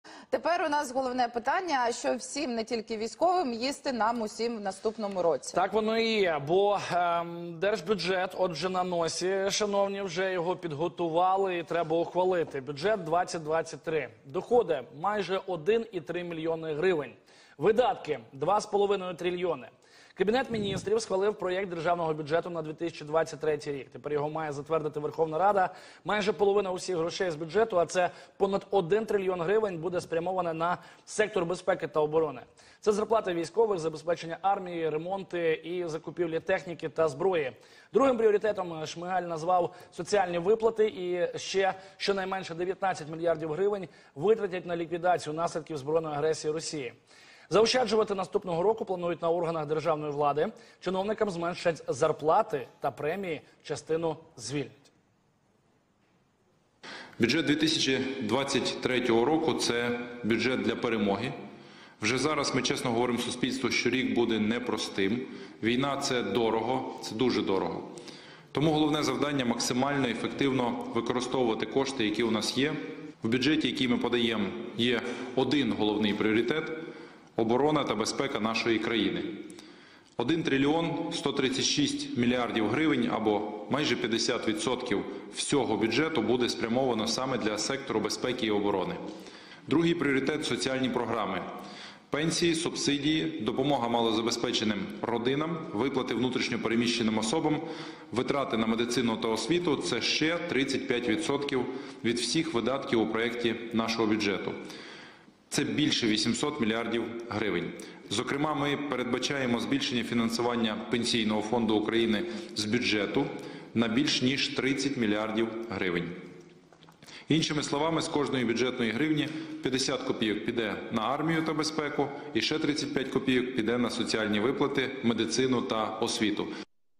"З кожної гривні 50 копійок на армію": Кабмін ухвалив проєкт бюджету-2023
Кабінет міністрів на засіданні 13 вересня затвердив проєкт бюджету на 2023 рік. Про це повідомив прем'єр-міністр України Денис Шмигаль.